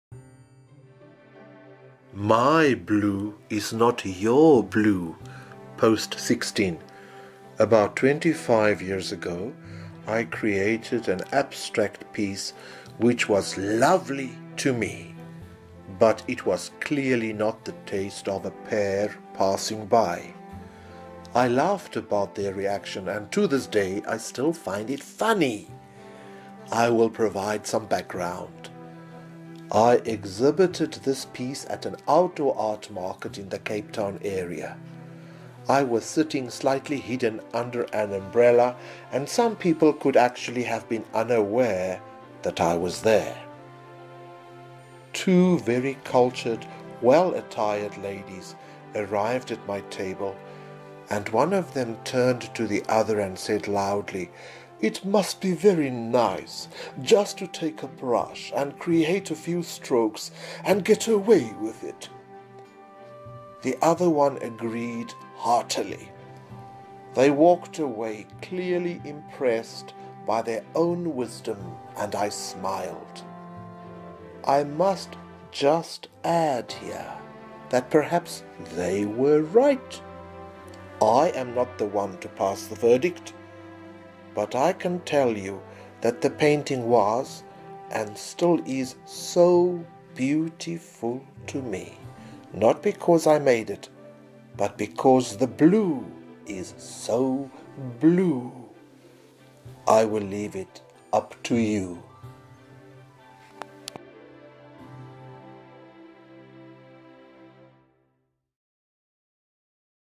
Dr P's Fun Factory audio post 16 "My blue is not your blue" - P. A. Pienaar 2021
This is number 16 in a series of 100 posts I would like to make. I am reading the blog posts I made during 2020 and I now can add visuals too. It is my  intention to add colour and hopefully inspire (myself and) others too. Let me know how you experience it.